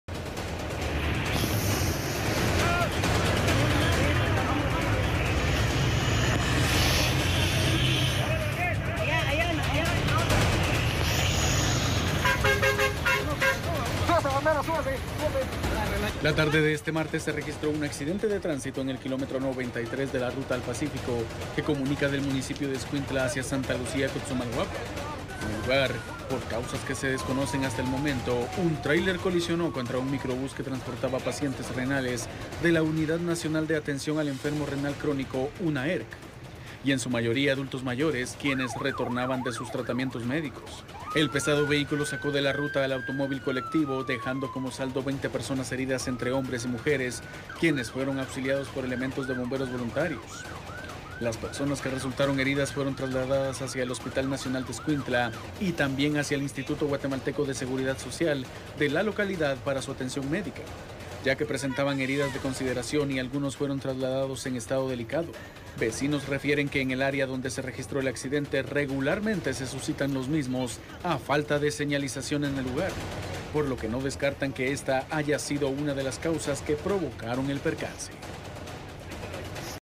Varios pacientes renales heridos en accidente de tránsito en Escuintla
Tráiler colisionó con microbús que transportaba a los pacientes

La colisión de un vehículo del transporte pesado y un microbús del transporte público dejó a varios pacientes renales heridos en Escuintla.